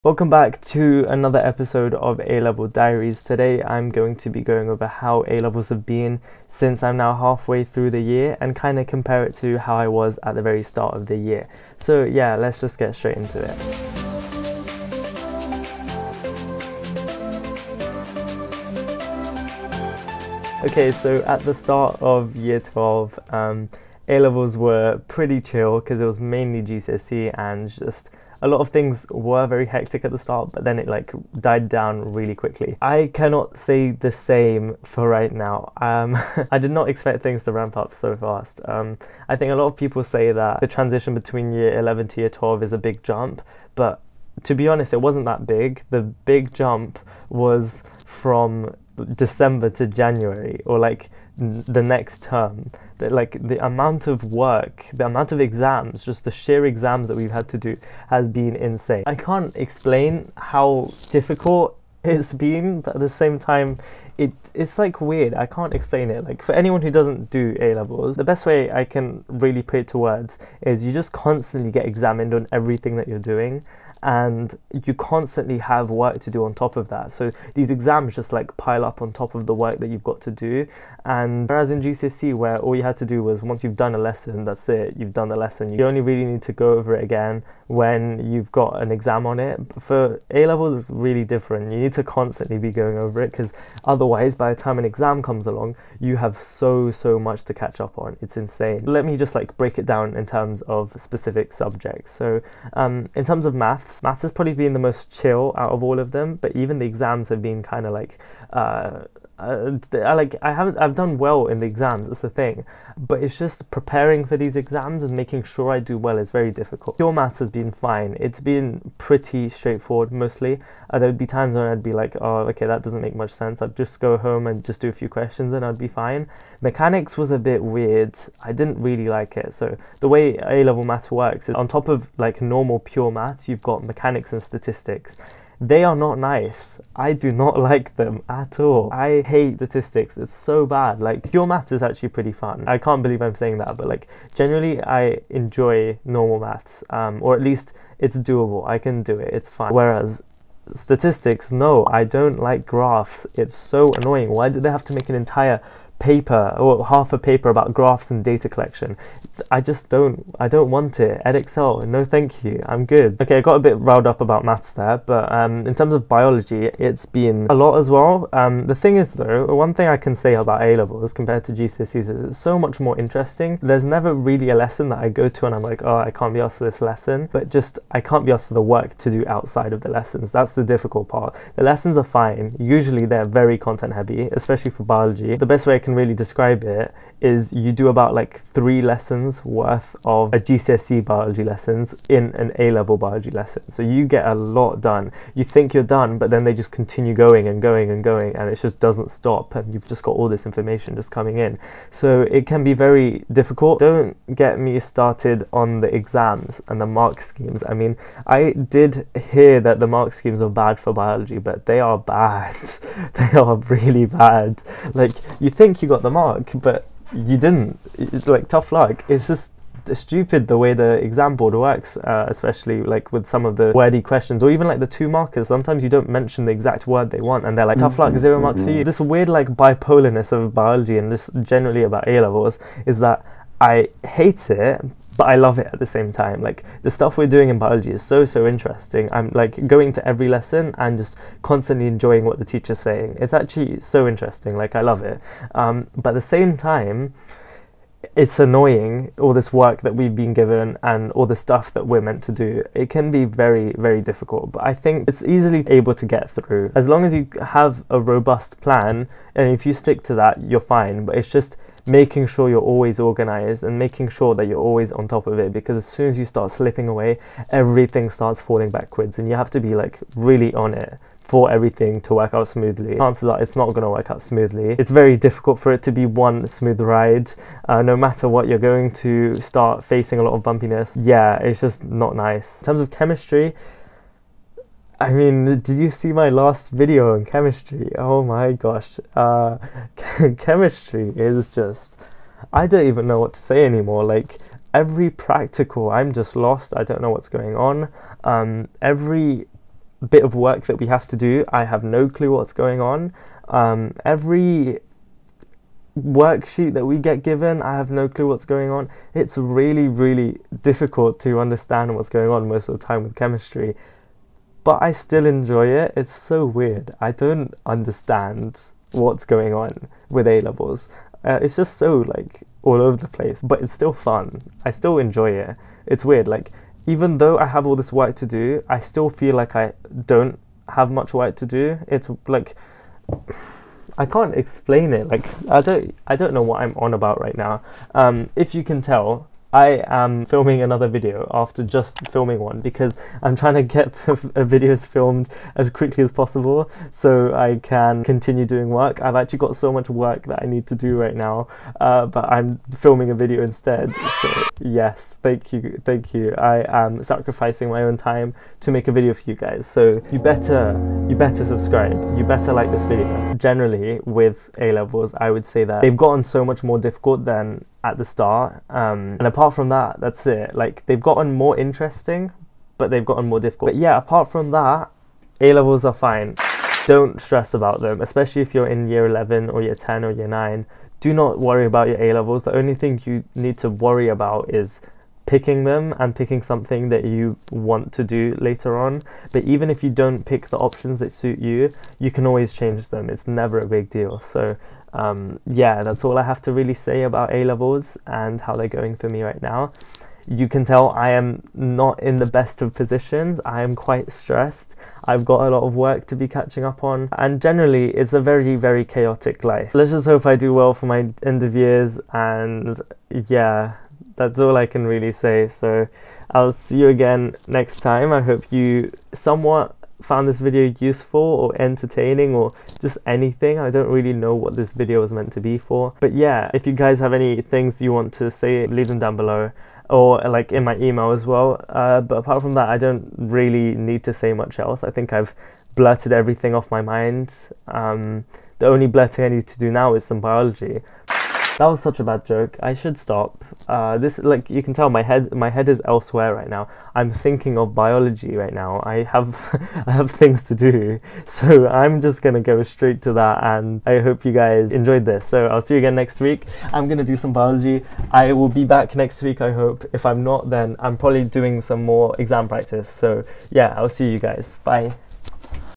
A-levels so far... halfway through Year 12 | A-level Diaries #3
Here's a bit of an update to my A-level series, and let me tell you, a-levels are no joke.

A-levels that I'm doing:
Maths [Edexcel] 🧮
Biology [AQA] 🧬
Chemistry [AQA]  🧪

GCSEs that I've done:
Maths [Edexcel] 🧮
Further Maths [AQA] 🧮  🧮
English Language [AQA] 🇬🇧
English Literature [AQA] 📖
French [AQA] 🇫🇷
Triple Science [AQA] 🧬  🧪  🔭
Geography [AQA] 🌏
Religious Studies [AQA A] ✝️  ☪️
Design Technology [Edexcel] 🔨